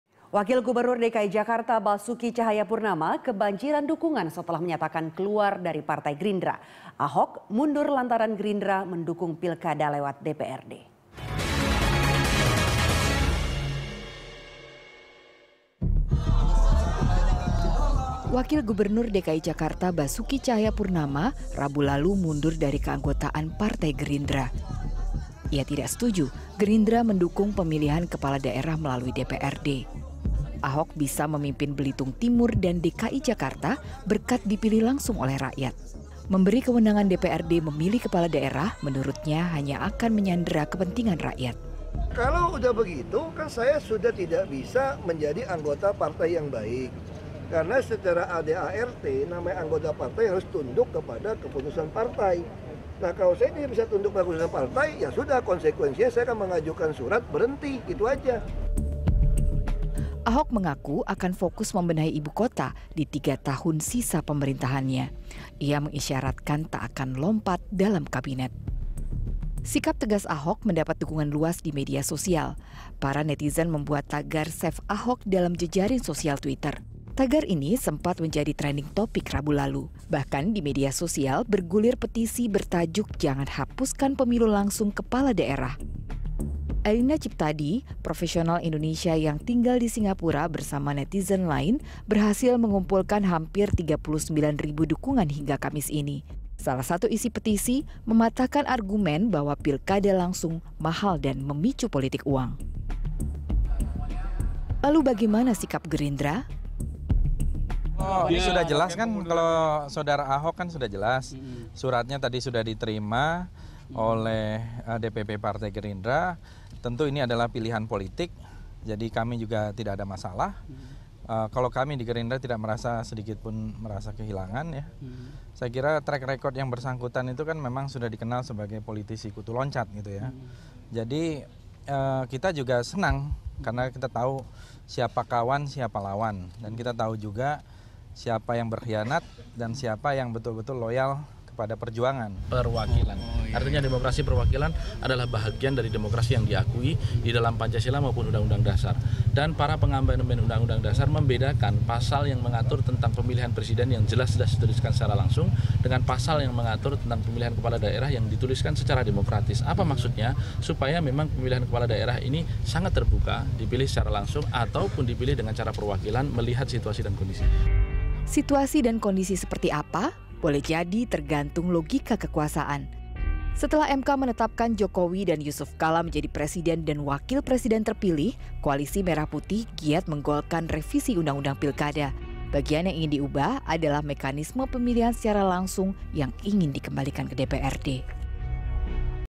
Ahok Mundur, Tolak Pilkada Lewat DPRD
Wakil Gubernur DKI Jakarta Basuki Tjahaja Purnama kebanjiran dukungan setelah menyatakan keluar dari Partai Gerindra. Ahok mundur lantaran Gerindra mendukung pilkada lewat DPRD.

Facebook.com/BeritaSatuTV
Youtube.com/BeritaSatu